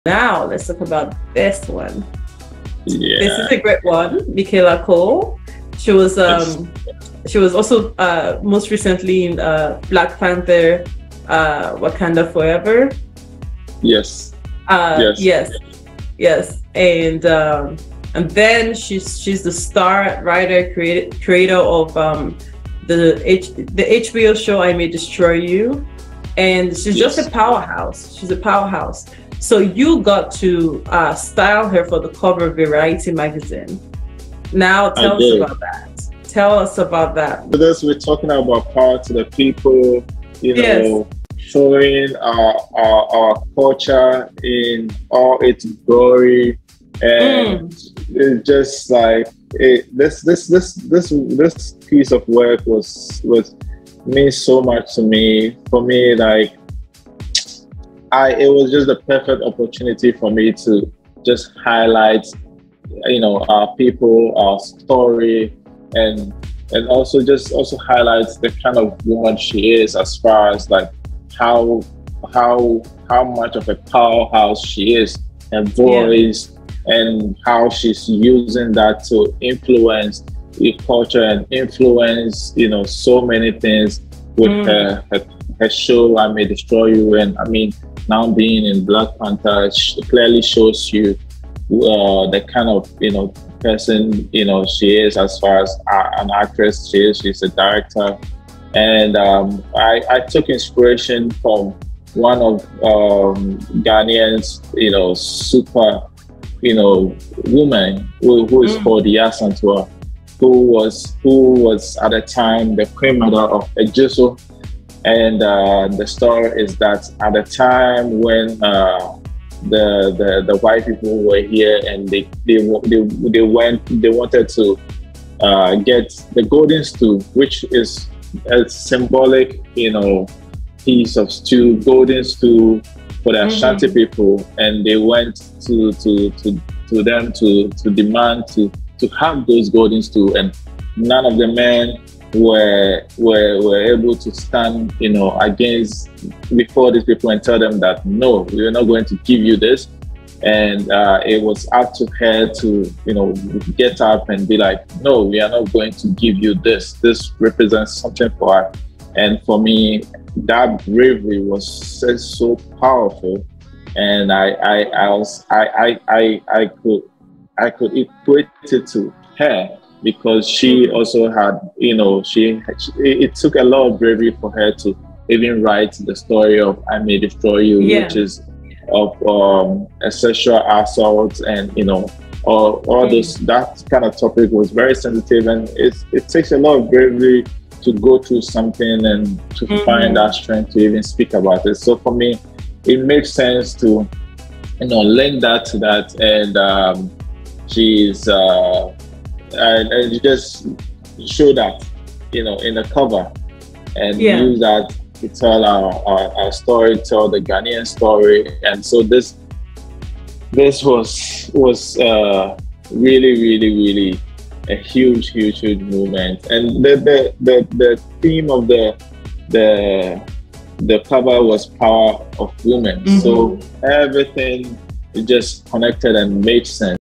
The Story Behind Styling Michaela Coel For Variety
Charting the Creative Journey of Nana Kwasi Wiafe - The Fashion Model, Stylist and Creative Director Transcending Ancient Ashanti Culture. TANTV with Nana Kwasi Wiafe, the founder of the brand Very Ghanaian. Our conversation provides a sneak peek into his unique culture, the inspiration behind his versatile creations and his contribution to Beyoncé’s Black is King.